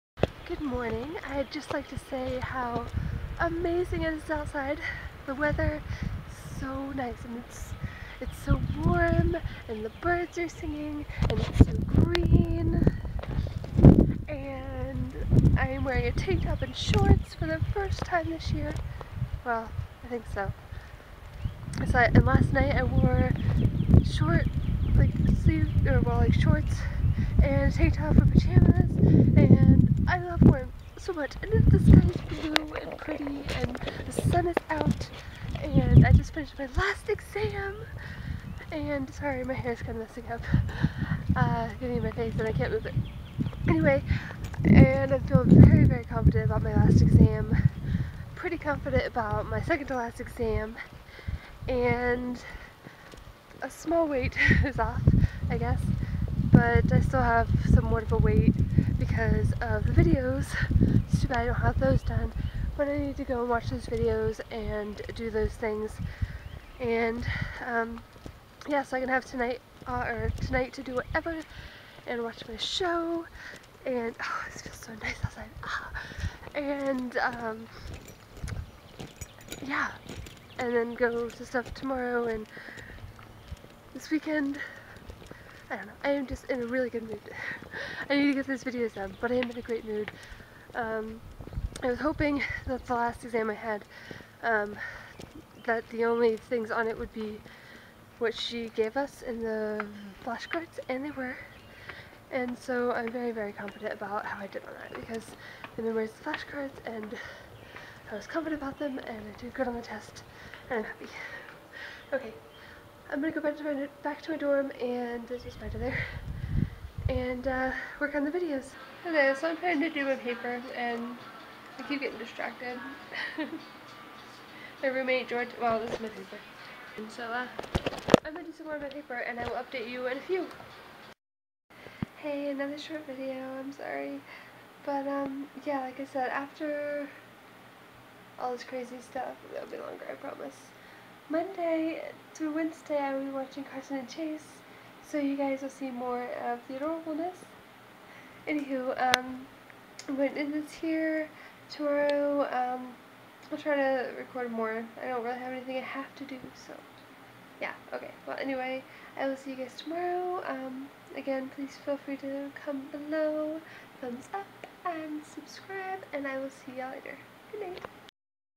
Getting Things Done (Day 124)
Finishing the last of my assignments. So sorry for the short video. Longer ones coming soon, promise! :)